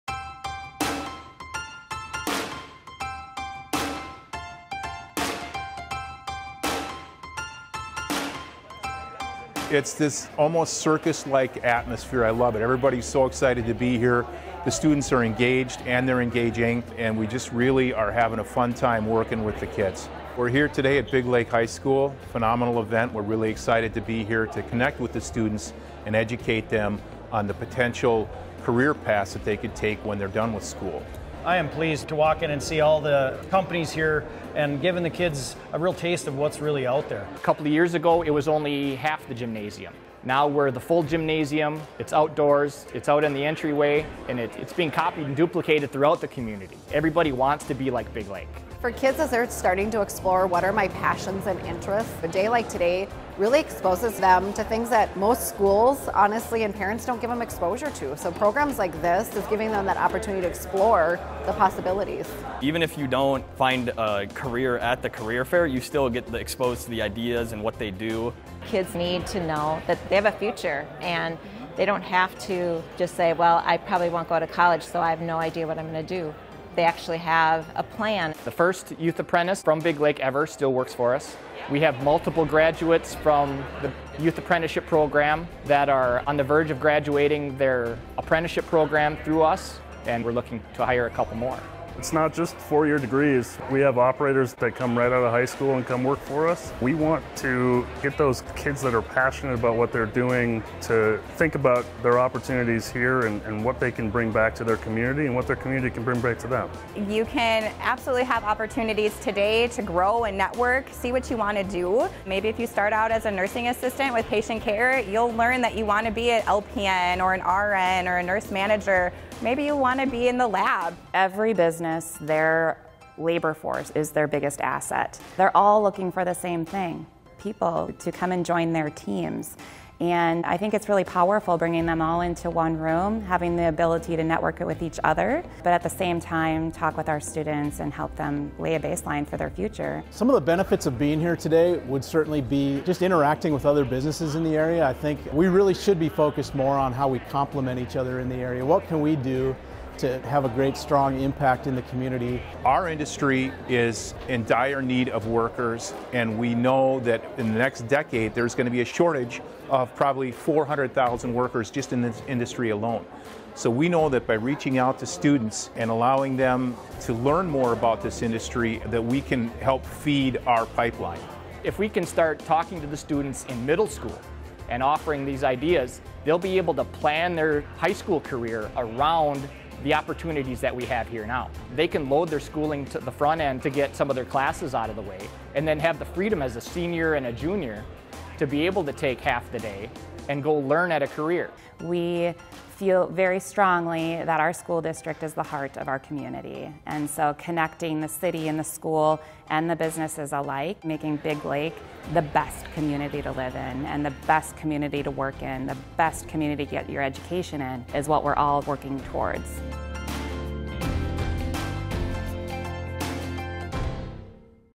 Big Lake Schools: Careers by Choice, Not By Chance
Careers by Choice, Not by Chance.

That is the motto at Big Lake High School. Through our nationally-acclaimed college and career readiness program, REAL, we are committed to providing lots of opportunities for our students to explore careers before they walk across the stage at graduation. This way, they can leave our school feeling confident that they have a plan for what is next.

We are so thankful to our many community partners that make our REAL program possible. Check out this video, from our career fair earlier this year, to see what they have to say about our amazing partnership!